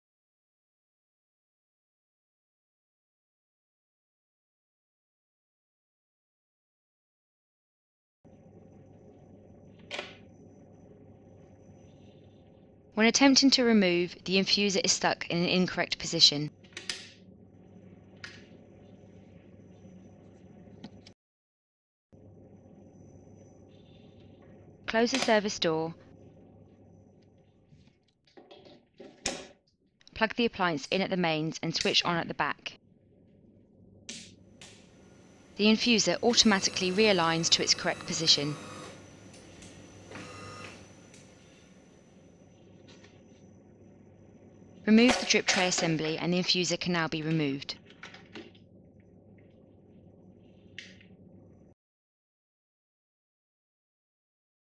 How to Reset a Stuck Infuser for Your De'Longhi ESAM Coffee Machine
Watch this short video to reset a stuck infuser in your De'Longhi ESAM coffee machine.

For more information on the De'Longhi bean-to-cup coffee machine